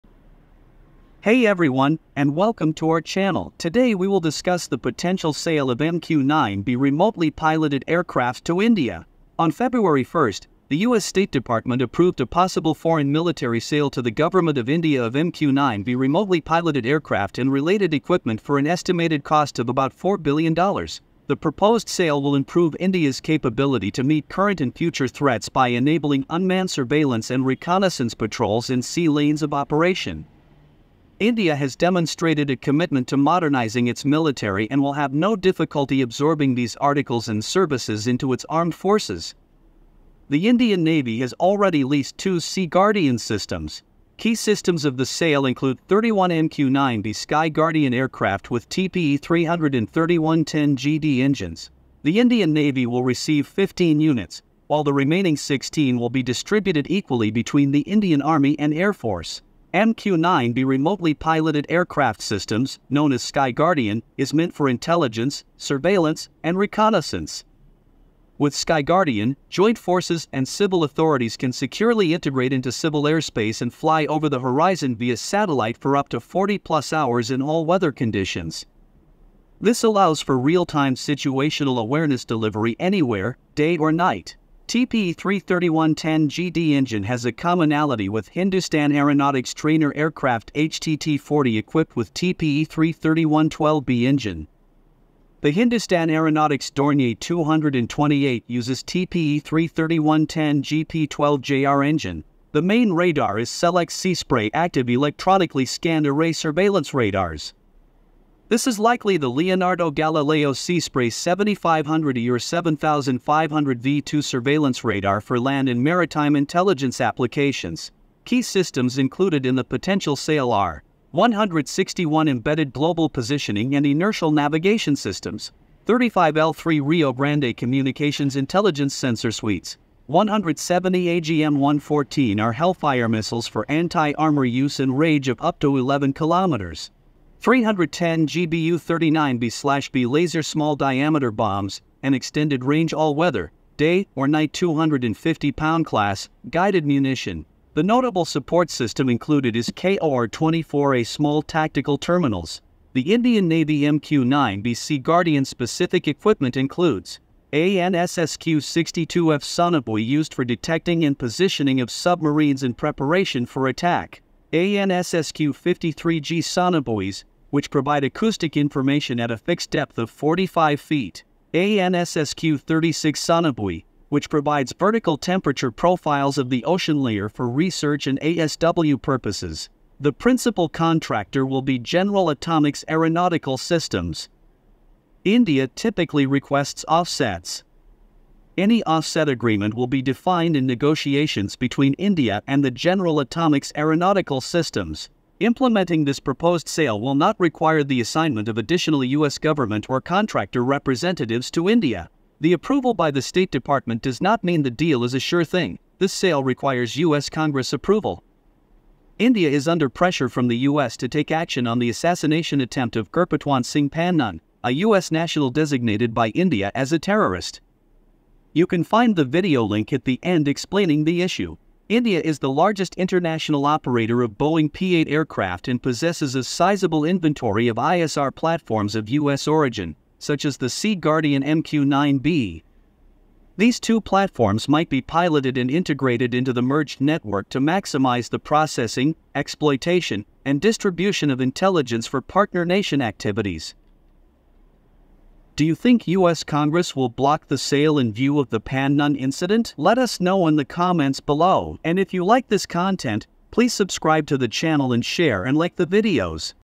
$4 Billion MQ-9B Drone Sale to India Approved by US - Congress to Review Deal Amid Pressure on India
The United States State Department has cleared a probable $4 billion sale of MQ-9B drones to India. The transaction would include 31 MQ-9B Sky Guardian drones, 15 for the Indian Navy and 8 for the Army and Air Force. Drones will improve India's monitoring, reconnaissance, and strike capabilities. However, the agreement requires Congressional approval and is under review due to tensions over India's handling of a Sikh terrorist.
Note:AI Voice Used
Videos: US DoD